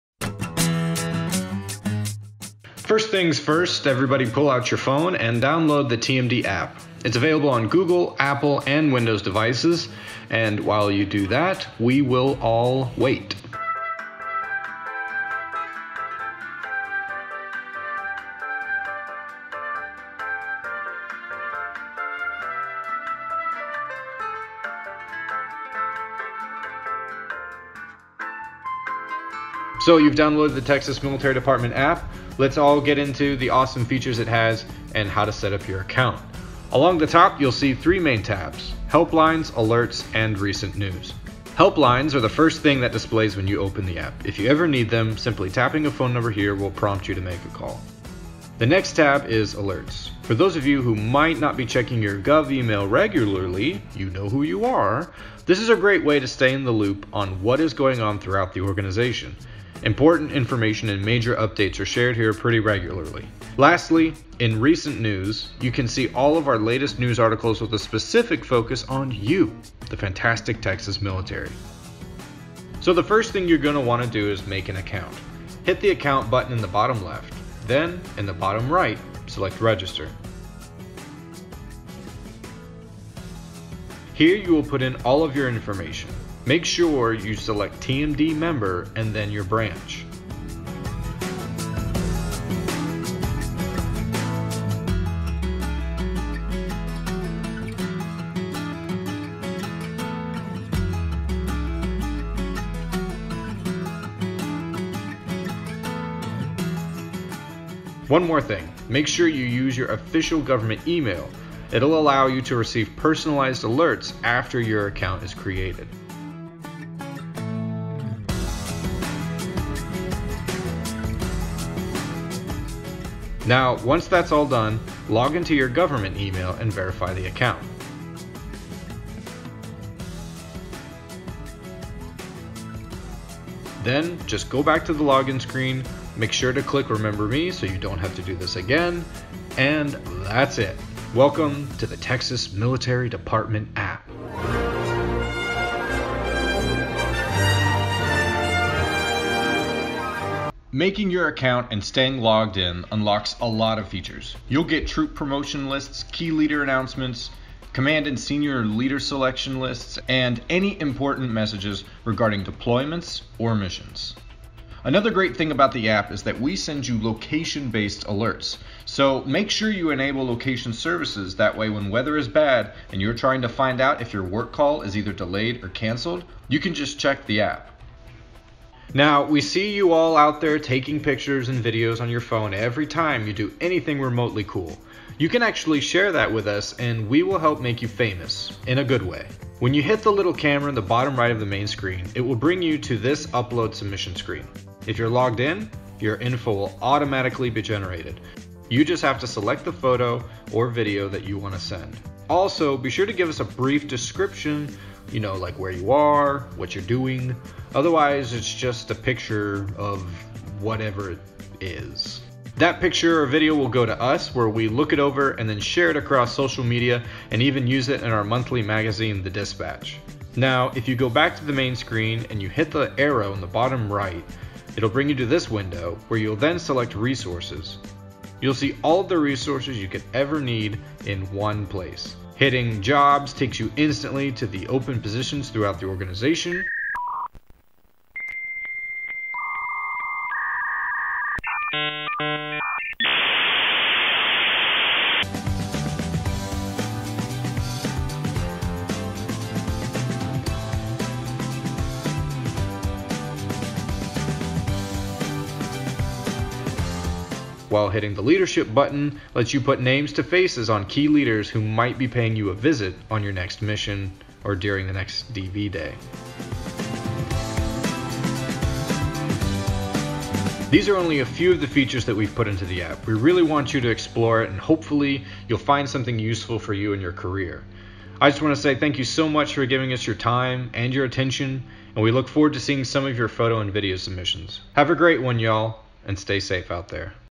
TMD App Tutorial
Instructional video on how to use the Texas Military Department App for Windows, iOS and Google devices